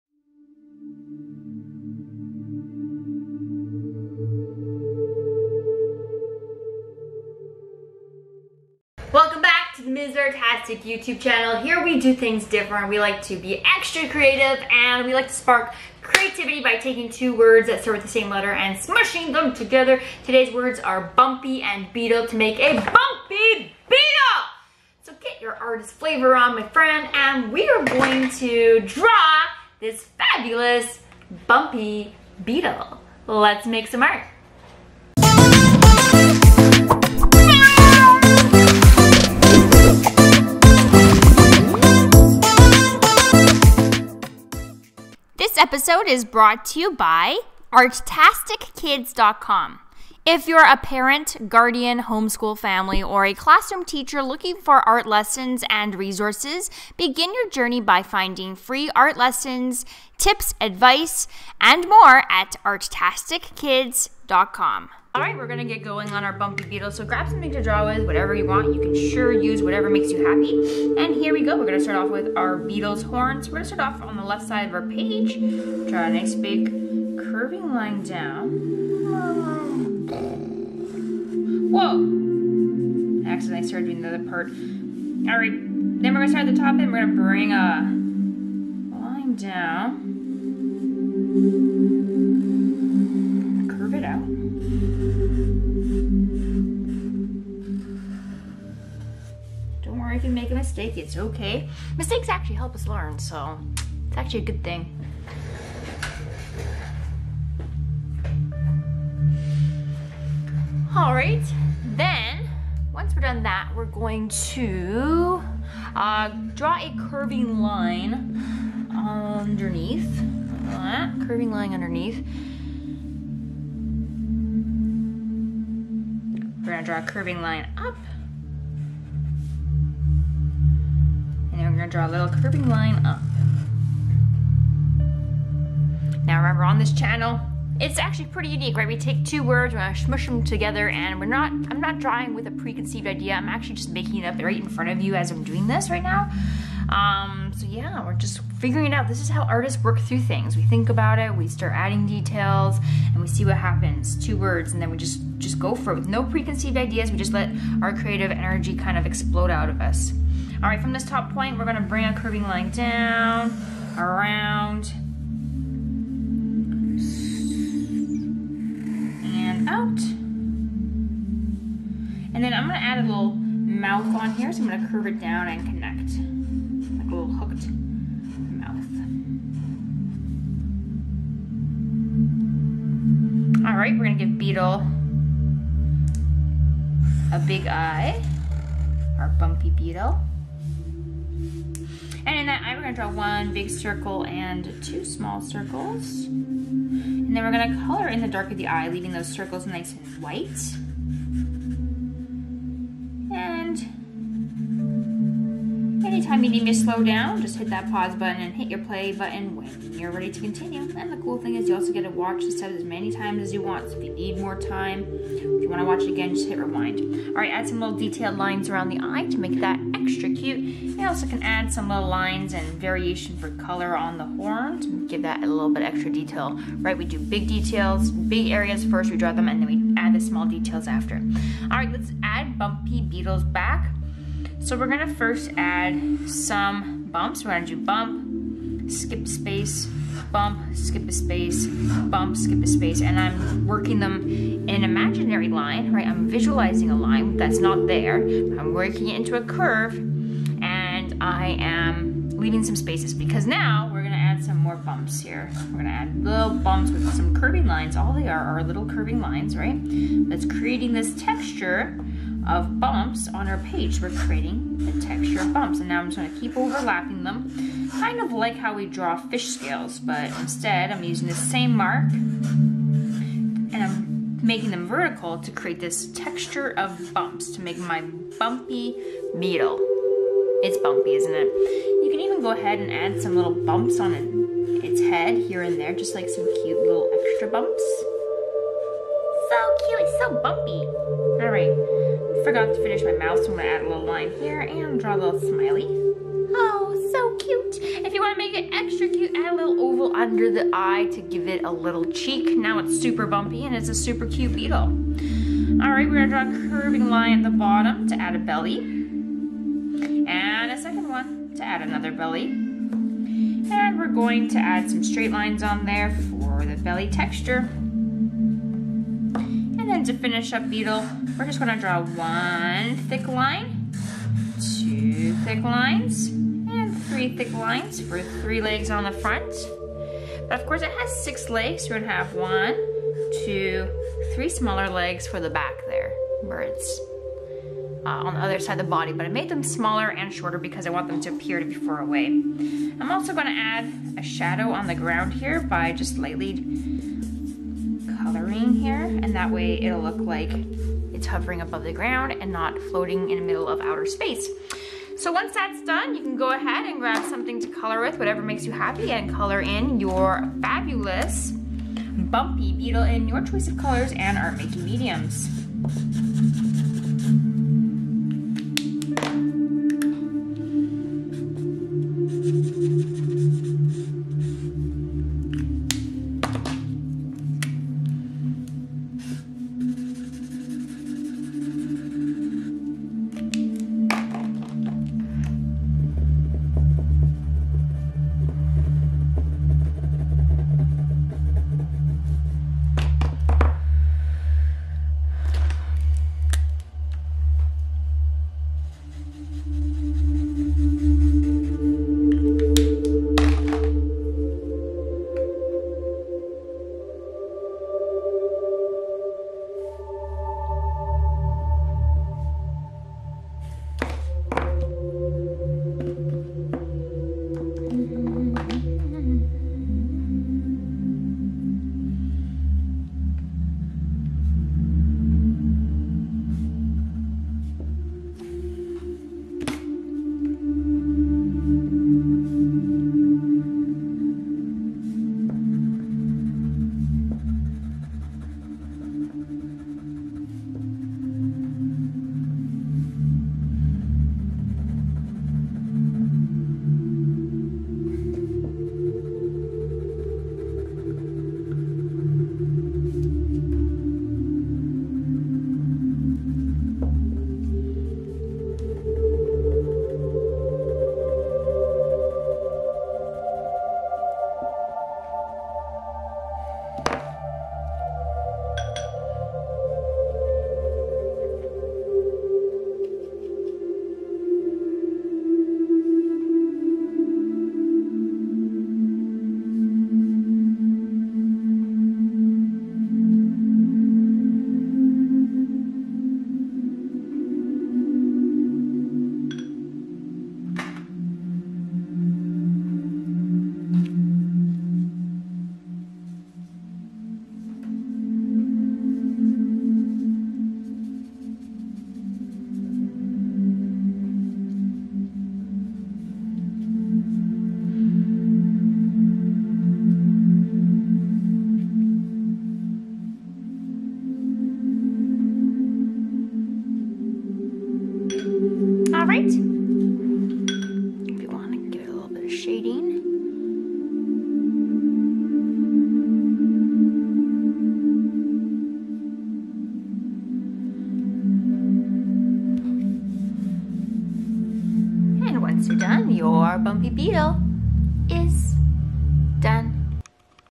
How to Draw a Bumpy Beetle for Kids!
Learn how to draw a Bumpy Beetle in this step-by-step tutorial. Whether you're an aspiring artist or just looking for a fun activity, this easy-to-follow guide will help you create your own Bumpy Beetle. From vibrant colors to intricate details, you'll discover everything you need to know to bring your Bumpy Beetle drawing to life. So grab your pencil and paper and get ready to explore and experiment making beautiful art!

4) Ms Artastic Blog | Art Education Resources & Tips: VISIT MY Art Education Blog